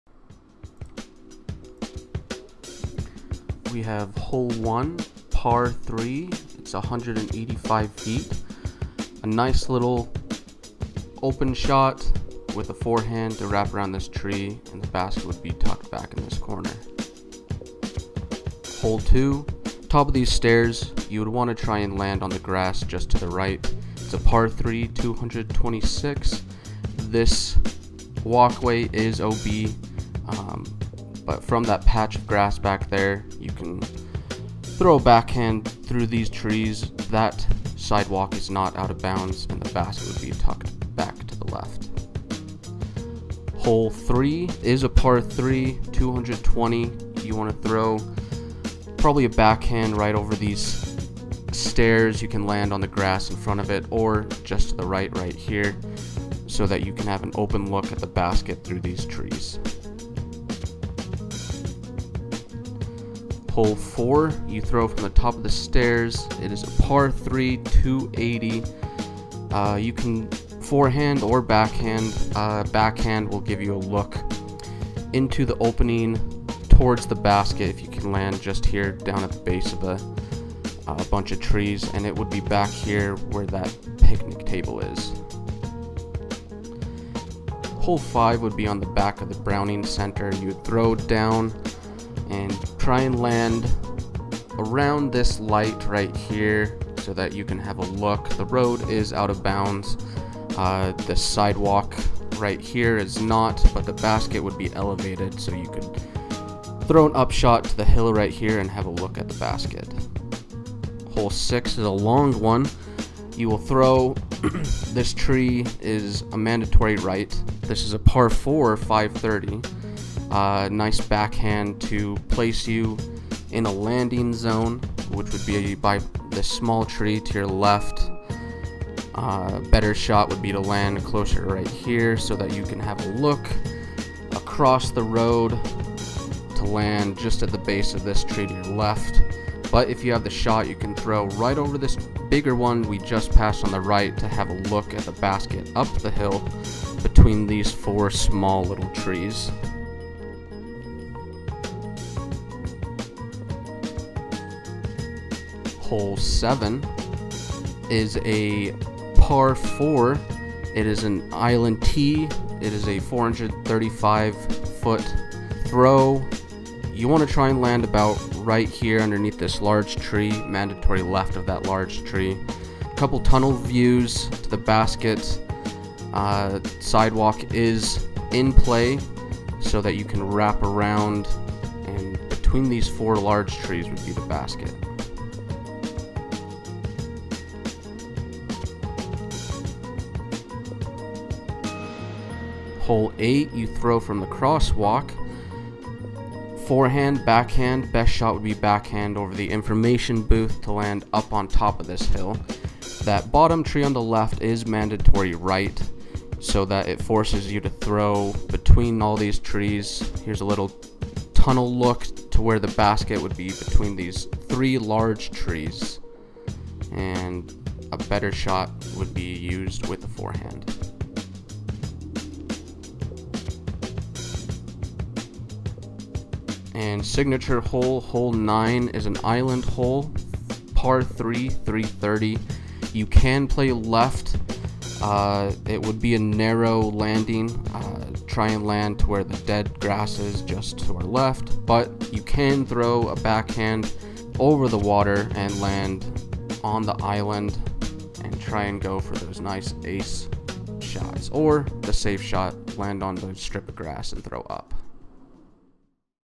What If - Weber State Campus Course
Hi everyone, this is a series I'm going around places and designing courses. I enjoy showing off my creativity, and looking at the areas around to see what could be used to make courses. This is an intermediate to advance course, and offers short layup shots and even those long accurate shots. This is all for fun, so let me know what you think of the course! Would you like to play it?